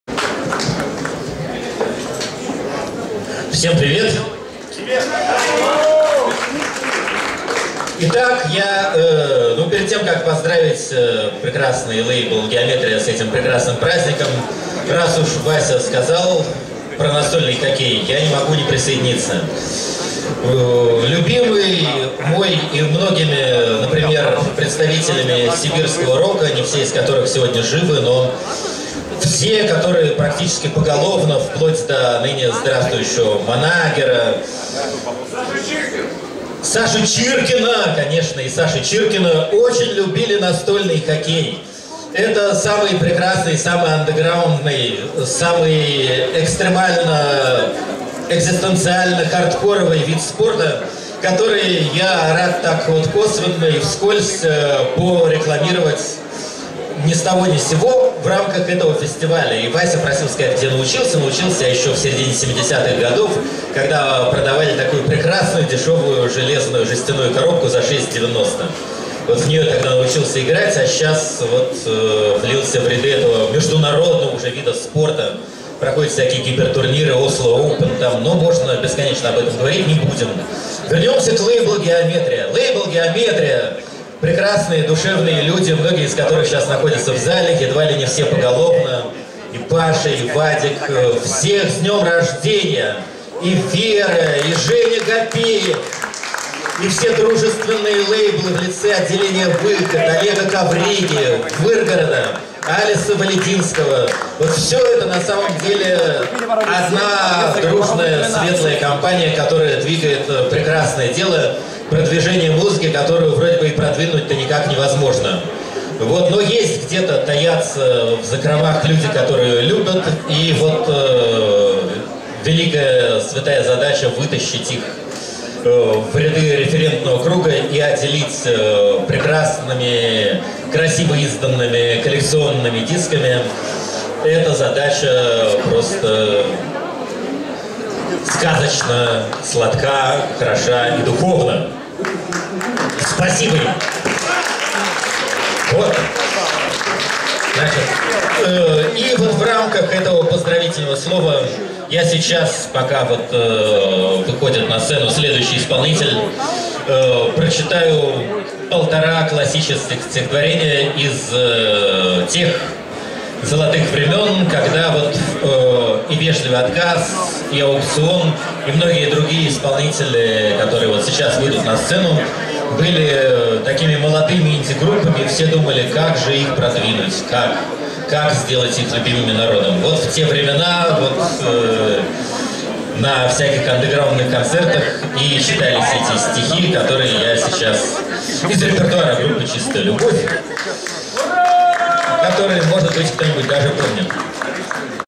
Сергей Гурьев поздр. Геометрию и рекл. наст. хоккей
2 декабря 2011 года Сергей Гурьев выступил на праздничном фестивальном концерте посвящённом десятилетию независимого музыкального издательства «ГЕОМЕТРИЯ» в клубе «Хлеб».